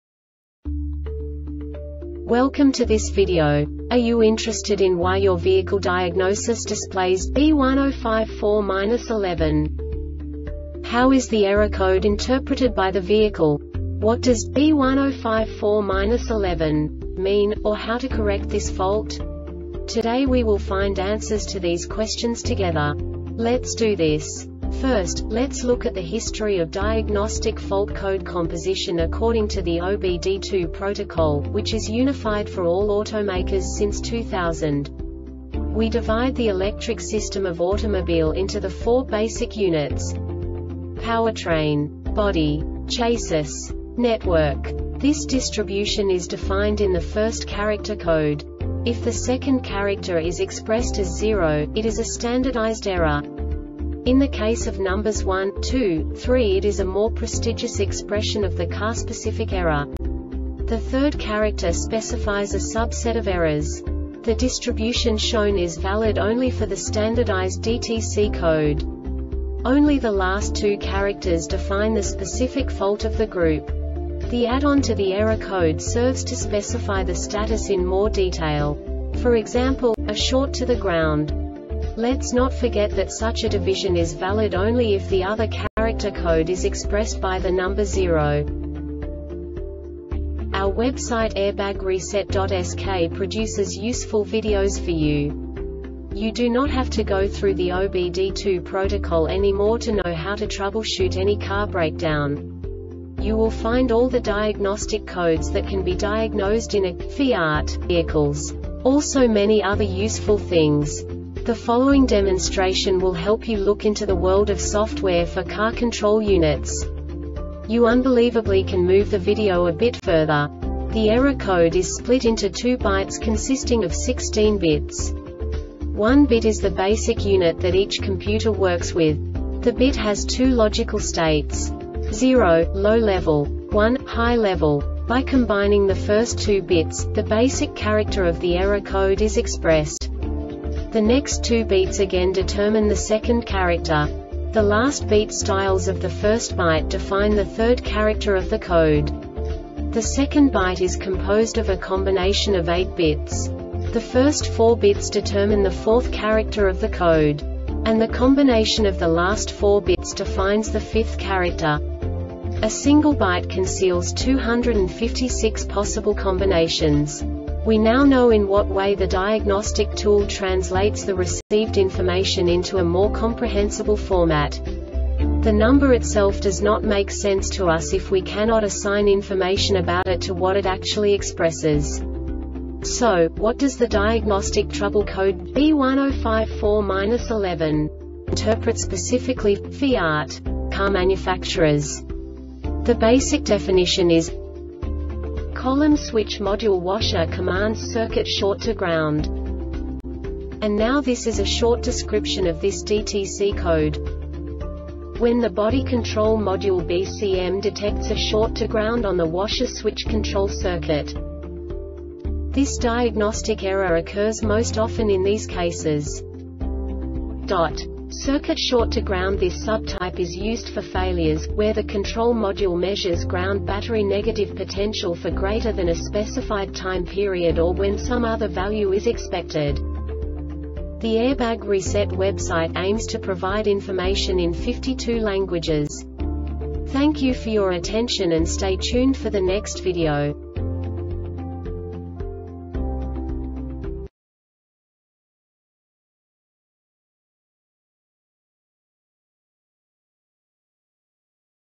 DTC Fiat B1054-11 Short Explanation
Contents:
0:21 Basic DTC analysis according to OBD2 protocol standard.
1:48 Insight into programming
2:58 A diagnostically understandable description of the Diagnostic Trouble Code
3:05 Basic error definition

Make:
FIAT
Code:
B1054-11
Definition:
Column Switch Module Washer Commands-Circuit Short To Ground
Description:
When the Body Control Module (BCM) detects a short to ground on the Washer Switch Control circuit.